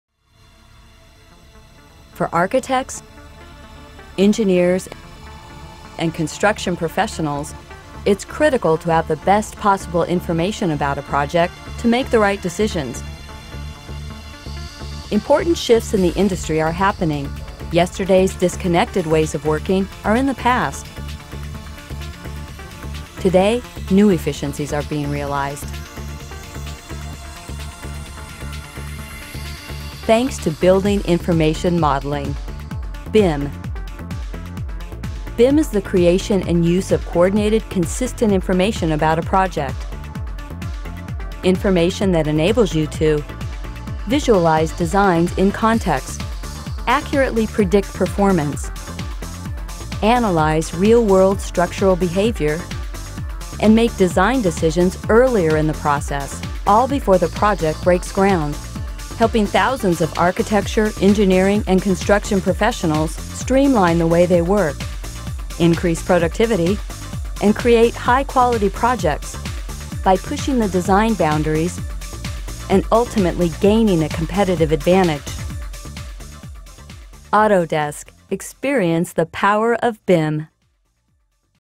BIM ile Yüksek Verimli Projeler. Ürün Demosunu Deneyin!
BIM'in, mimarlar, mühendisler ve yapı profesyonelleri için getirdiği avantajlar hakkında bilgi sahibi olun.